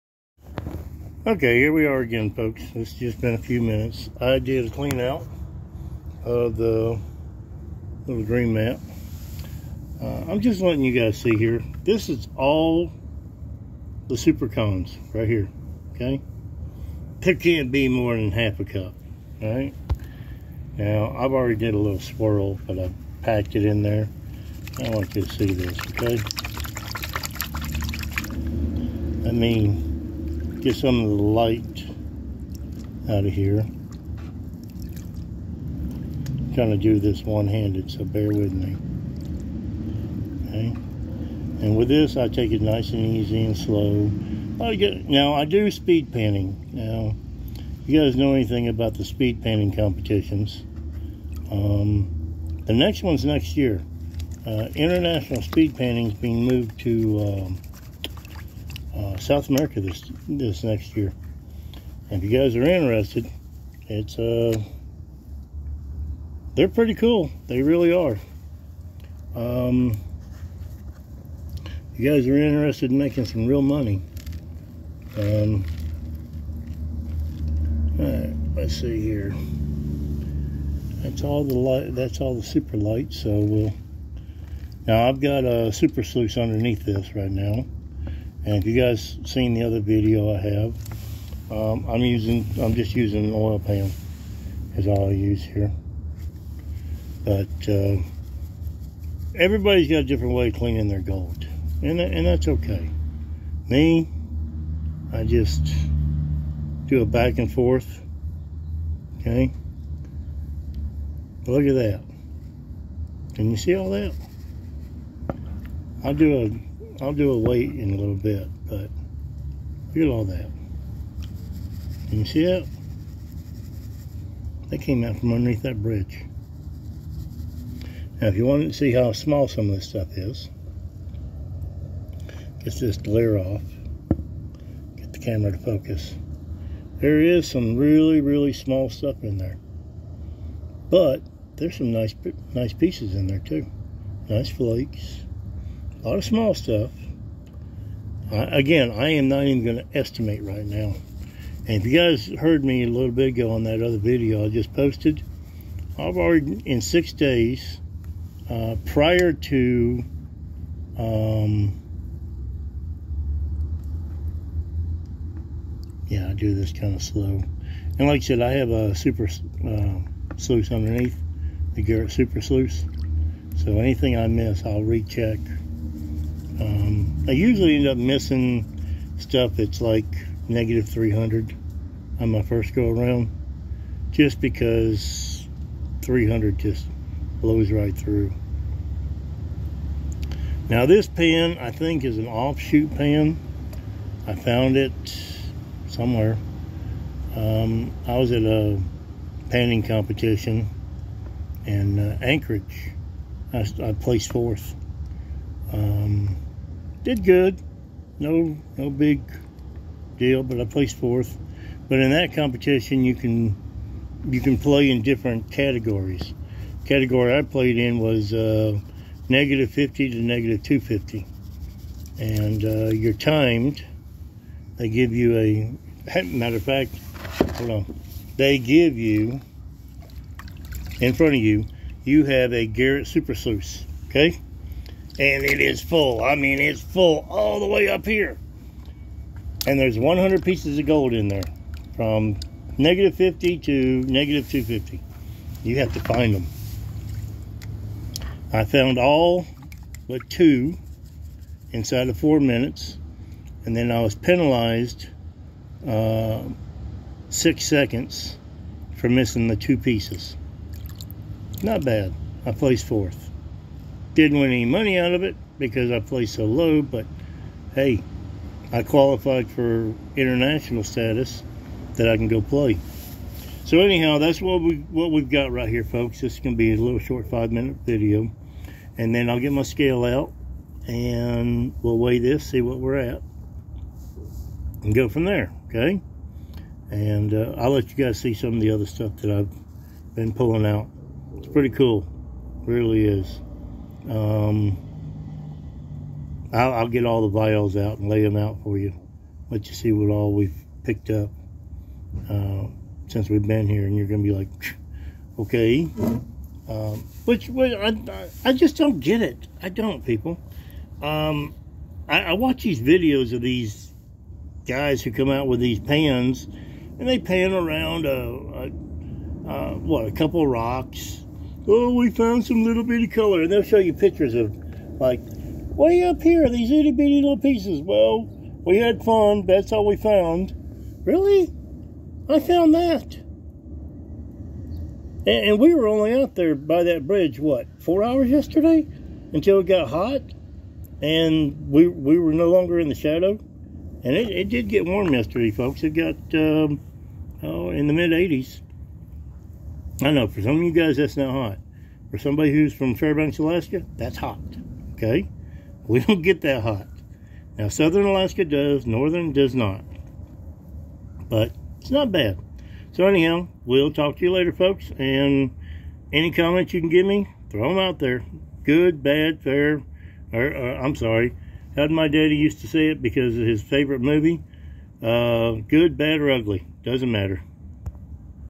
Panning out the "Cons" from after running A Dream Mat Sluice system
Just a short video of me panning out the "Cons" from The Dream Mat Sluice System.
barely half a cup is usually all you end up with.
I'll post a weights video of all 6 days in just a bit